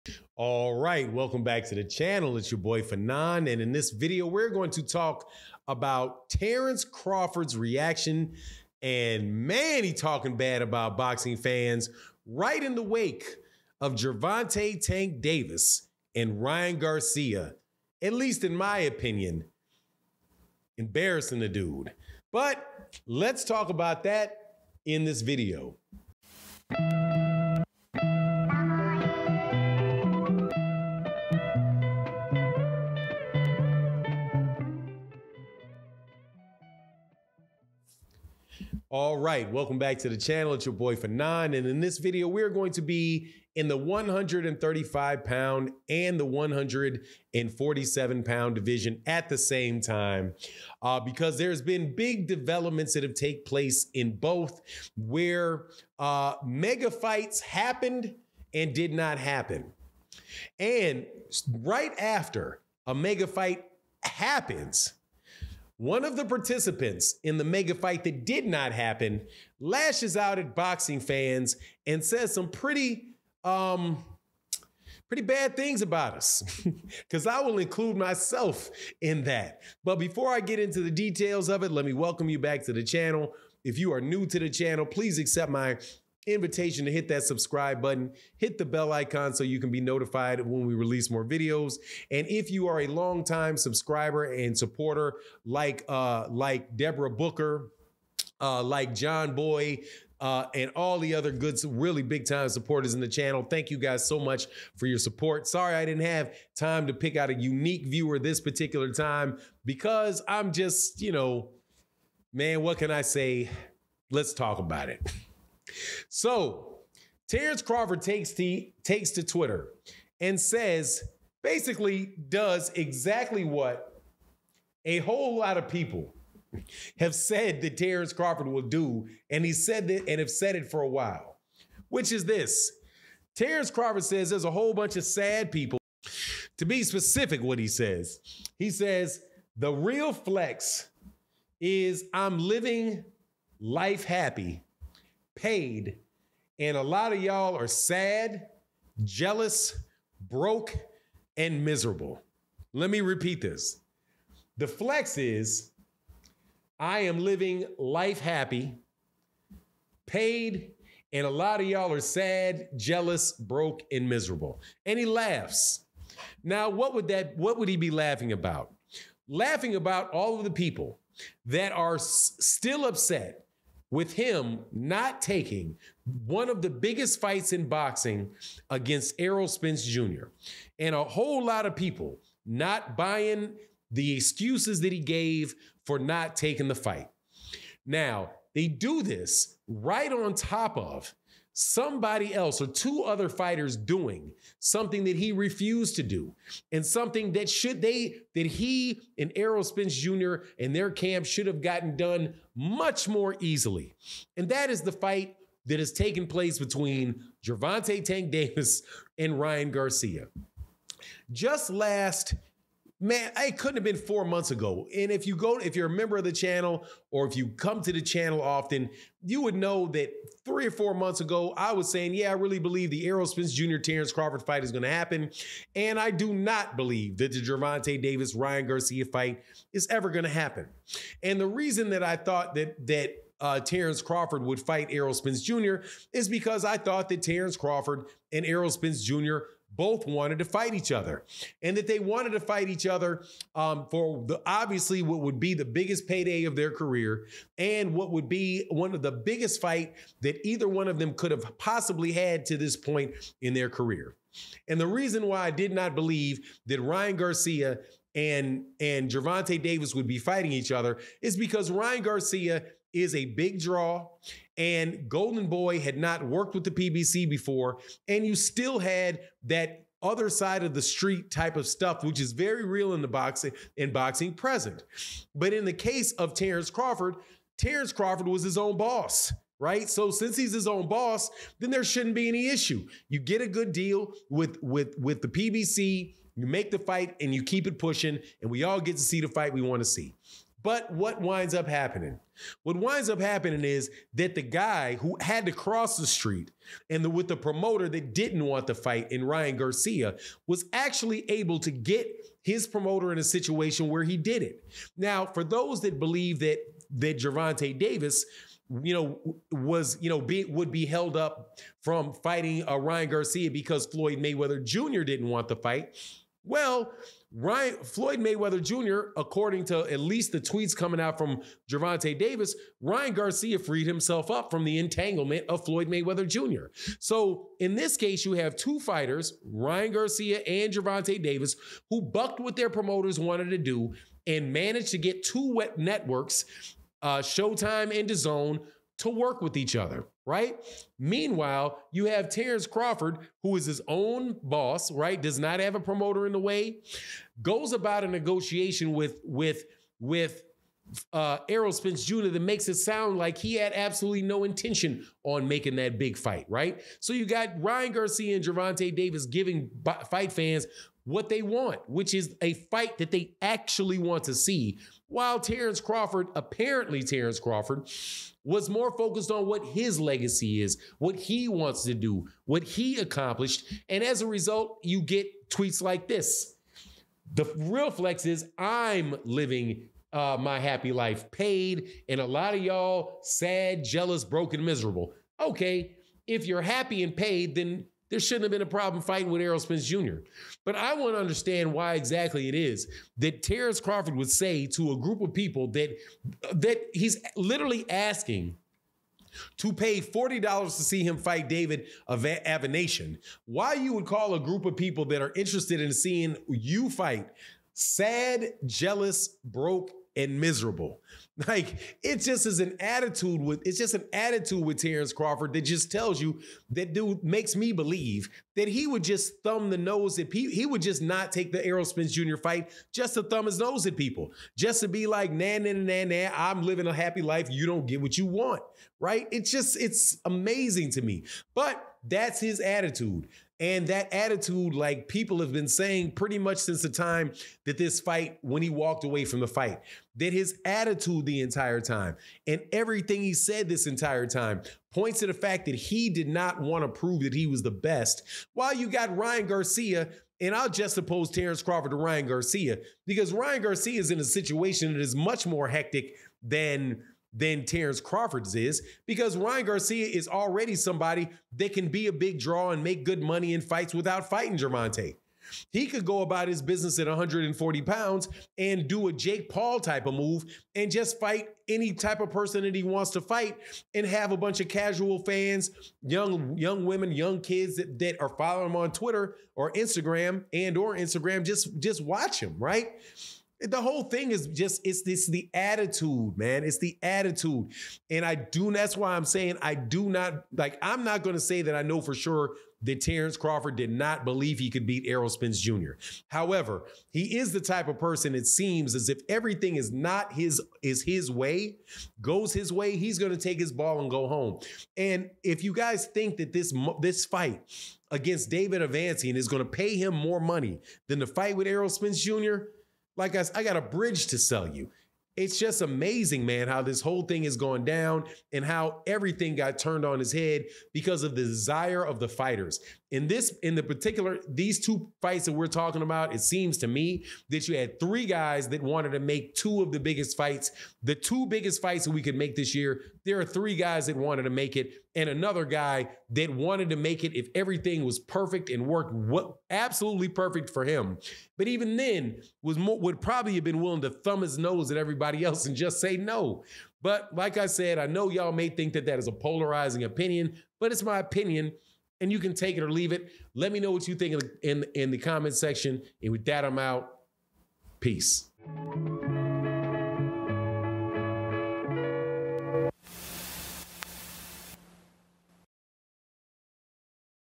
WHOA! TERENCE CRAWFORD LASHES OUT AFTER GERVONTA DAVIS AND RYAN GARCIA ANNOUNCED! BLASTS UPSET FANS!
TERENCE CRAWFORD LASHES OUT AT BOXING FANS AFTER GERVONTA DAVIS & RYAN GARCIA MAKE IMPROBABLE FIGHT!